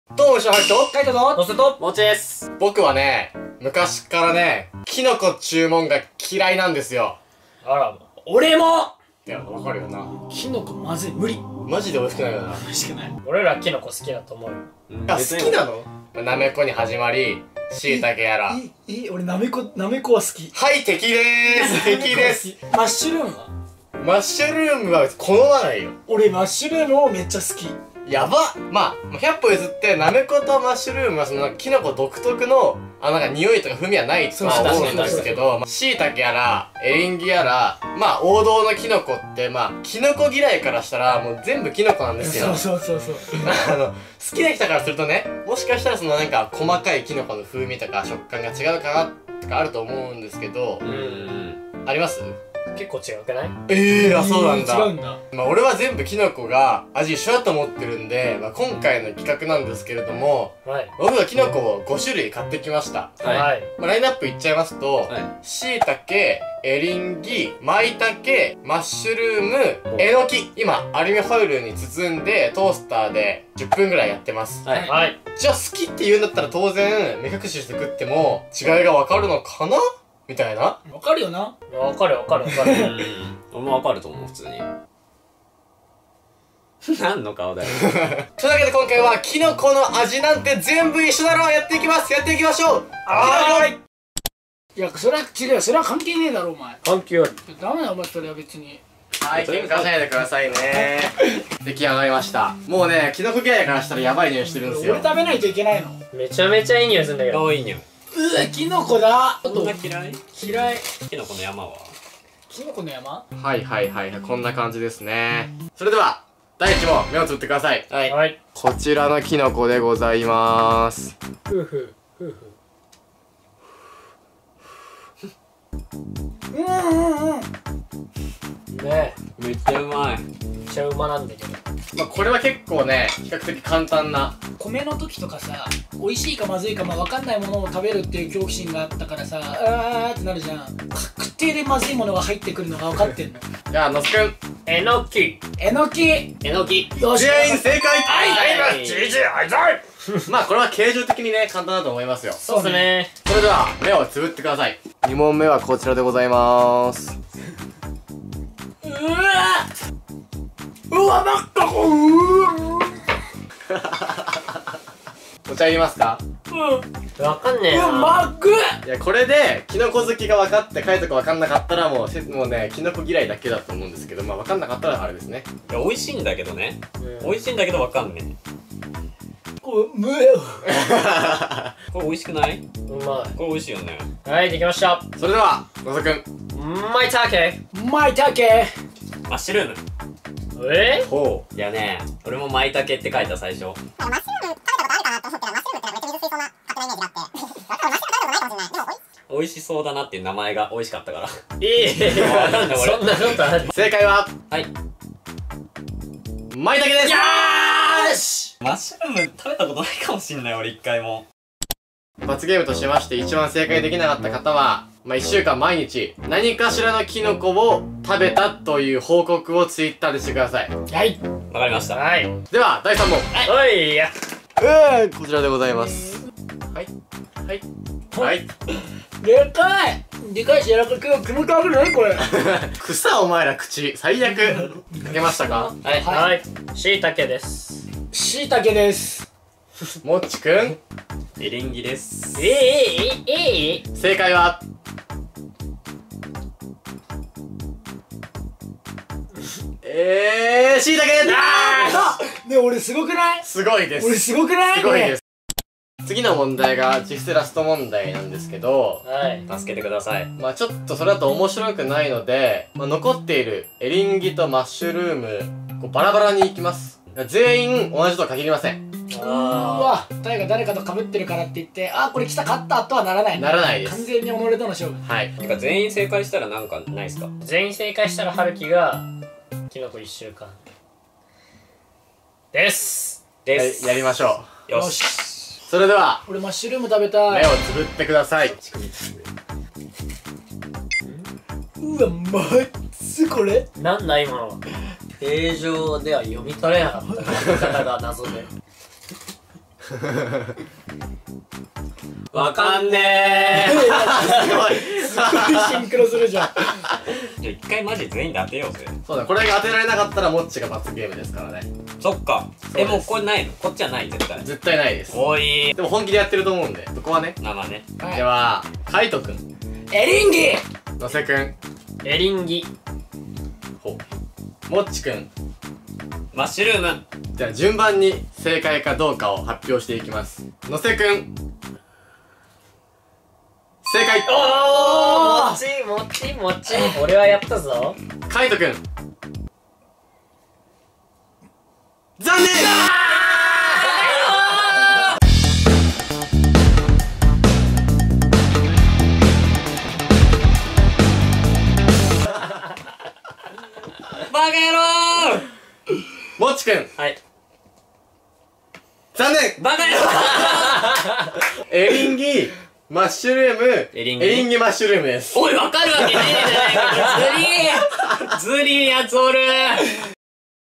【栽培】きのこの味なんてどれも一緒だろ！！！
皆さんはきのこお好きでしょうか。僕は小学生ぐらいの頃にきのこ嫌いが発覚して以来、給食の時間が純粋に楽しめなくなりました。あれは小学三年生のころだったでしょうか。副菜に「なめこのおろし和え」というなめことほうれん草、にんじんが大根おろしで和えてあるという地獄の料理が出されました。今思い返してもあの料理を給食に出す栄養士のかたの気がしれません。僕はなめこにトラウマを抱えていたのでいかにその副菜を食さずに給食の時間を乗り切るかを考えました。そこで僕が思いついた作戦はこうです。まず箸を持ちます。そしてその副菜の右斜下ぐらいに直径1cmぐらいの穴を開けます。そしれ押しのけられた野菜たちをを周りの野菜ににぎゅっぎゅっと押しつけ質量が減ったように見せます。この作戦を取ることで担任の先生は「あ、この子は苦手なりに一口は食べたんだな。偉いな。」と食べてないことをごまかせるだけでなく、他の生徒より評価があがるという魔法のような効果が得られます。さあ給食を片付ける時間がきました。配膳台に並びます。目の前には先生が。僕は素知らぬ顔で副菜の乗った皿を配膳台に戻します。すると「はるきくん、一口は食べなさい。」

薮崎先生、俺はお前を許さない。

【コンセプト】
❏何気ない日常の異常に面白い瞬間」を僕たちと一緒に共有できたら最高です！

❏ 公式
❑ 晴生(はるき)
❑ 魁斗(かいと)
❑ のせ
❑ もっち
【Untitled_mo】